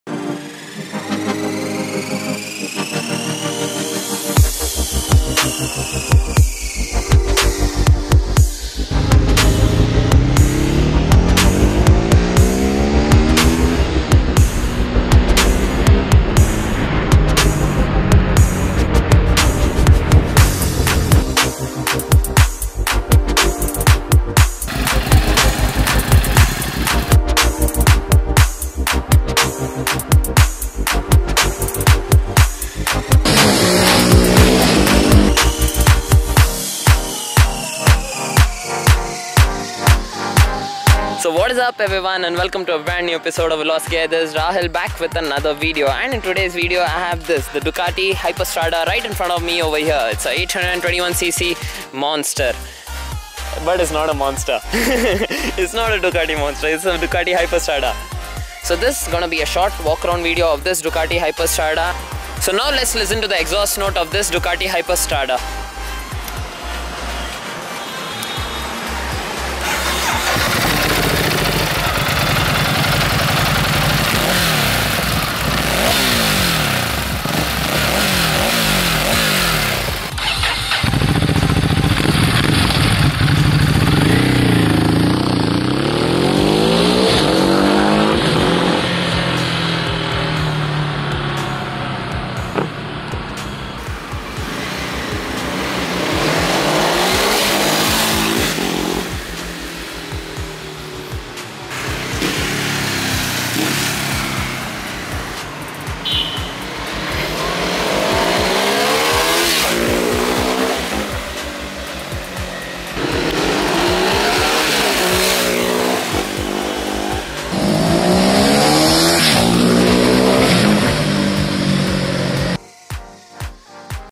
DUCATI HYPERMOTARD IN INDIA! | WALKAROUND | EXHASTNOTE | FLYBYS!
A WALKAROUND AND EXHAUST NOTE VIDEO OF DUCATI HYPERMOTARD IN INDIA, BANGALORE.